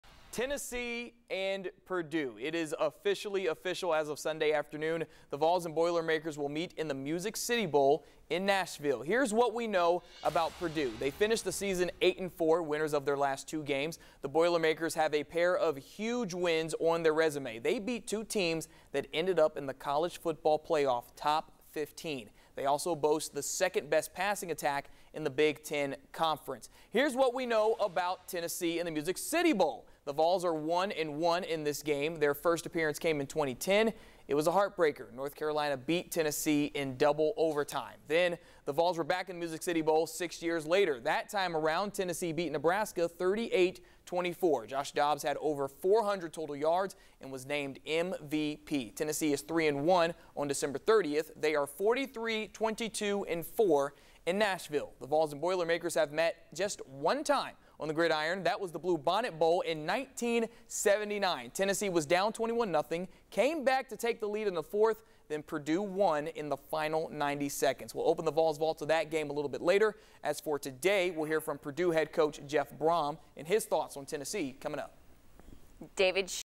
Tennessee Vols to face Purdue in the Music City Bowl in Nashville
Five of Tennessee's last seven bowl appearances have been against Big Ten opponents. They have won all five dating back to 2008.